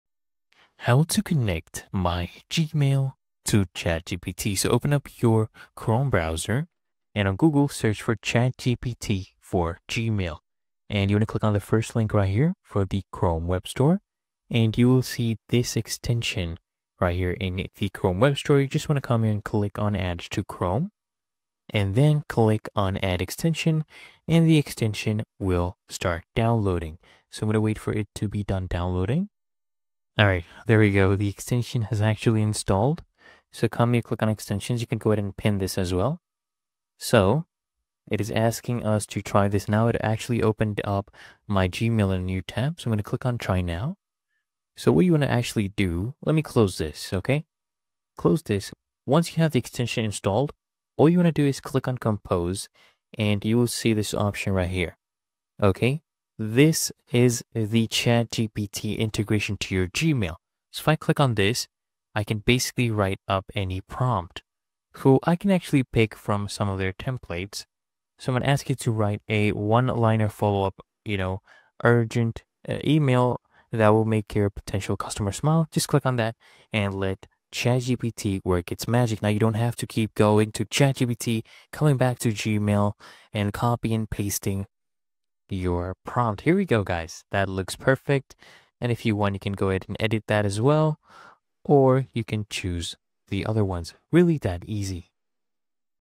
How to Connect Gmail to ChatGPT (Step by Step)
Learn how to easily connect your Gmail account to ChatGPT with this step-by-step tutorial. Enhance your email experience with AI-powered assistance!